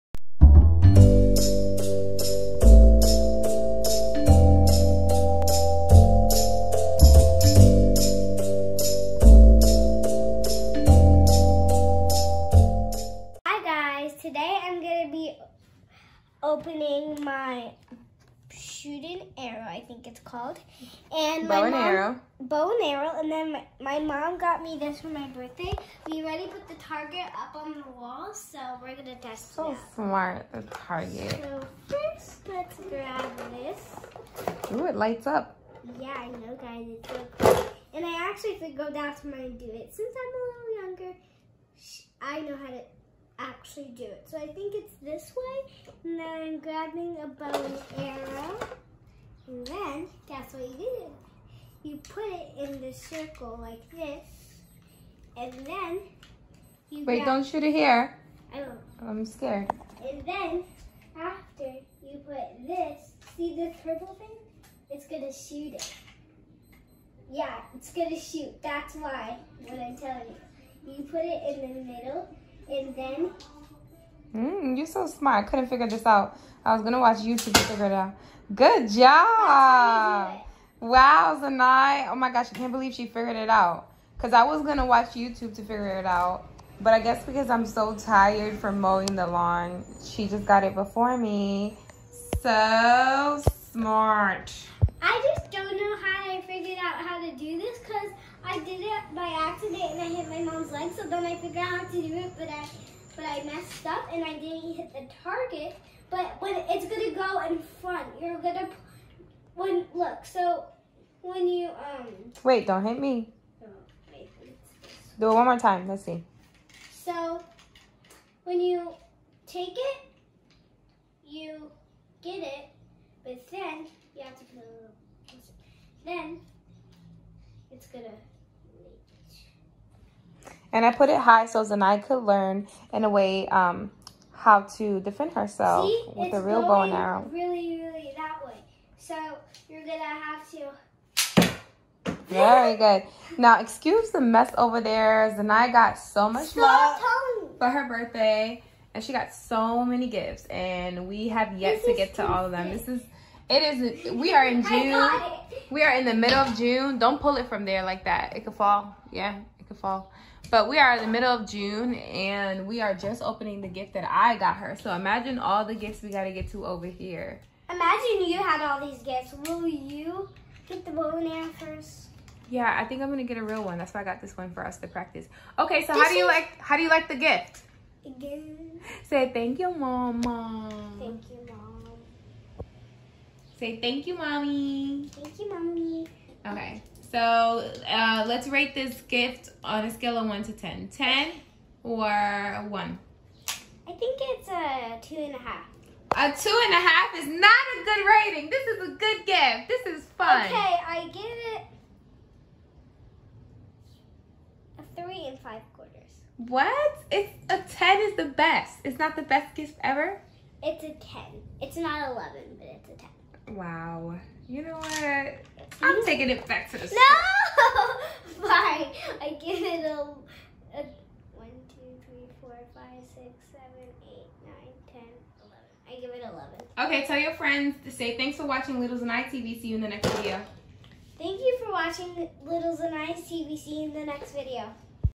Little Zanih TV | Zanih opens birthday gifts a month later
Zanih got some many gifts for her birthday but only opened 1 in June. Ask her to open more!